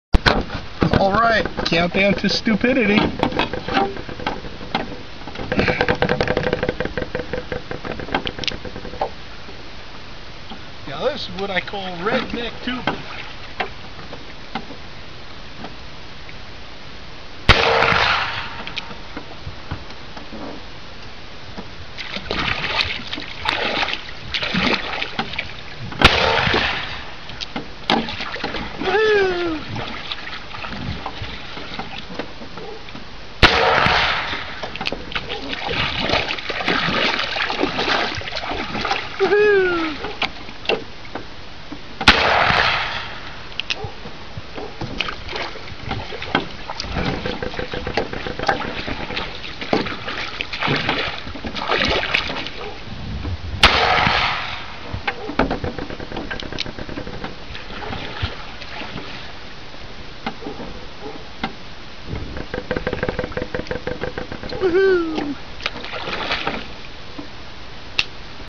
redneck tubing new Olympic sport
a new Olympic sport I invented somewhere around 2007, I just can't believe it hasn't caught on yet LOL